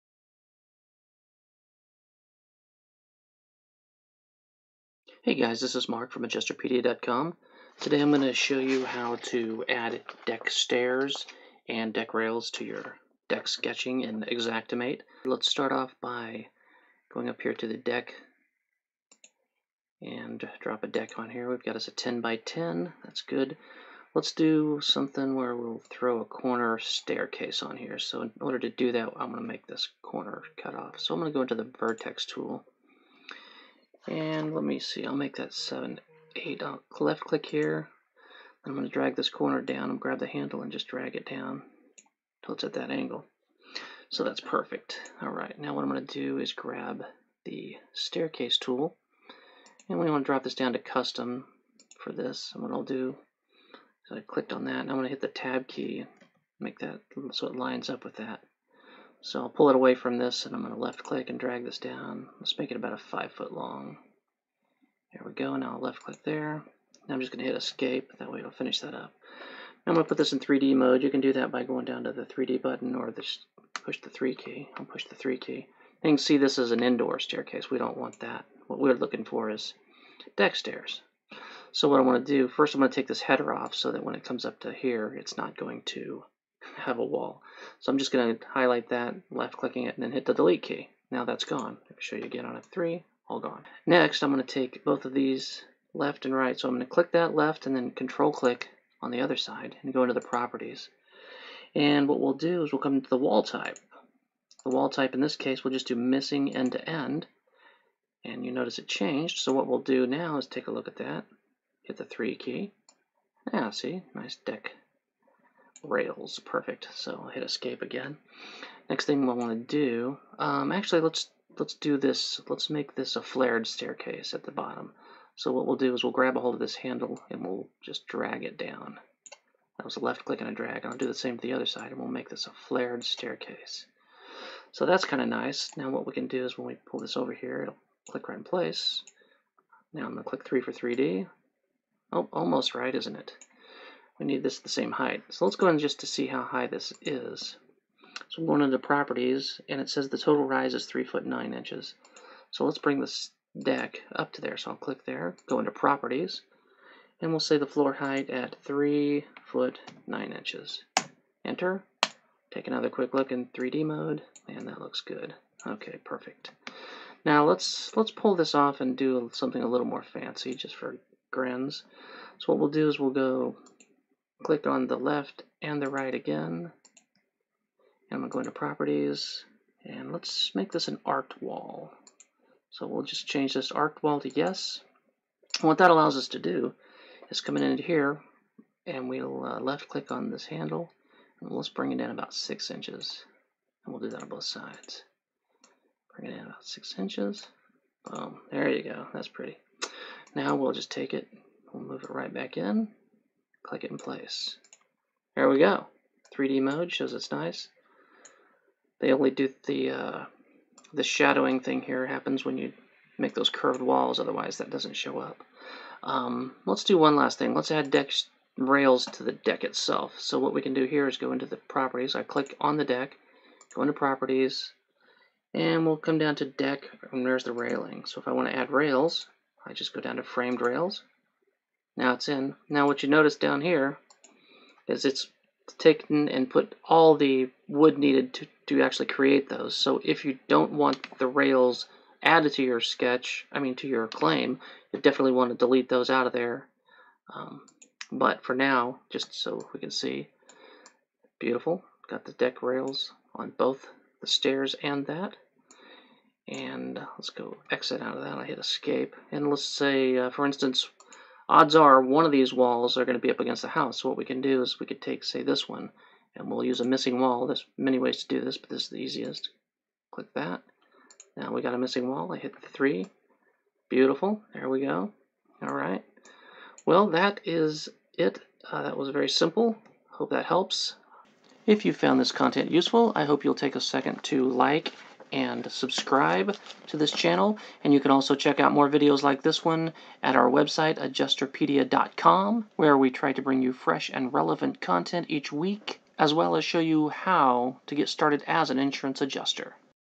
How to Add Deck Stairs and Rails in Xactimate
This Xactimate tutorial will show you how add deck stairs and deck rails to your sketch in Xm8. I also show how to add flared and curved stairs to your sketch.

for to learn more about insurance adjusting, xactimate tutorials, adjusting tools and much more.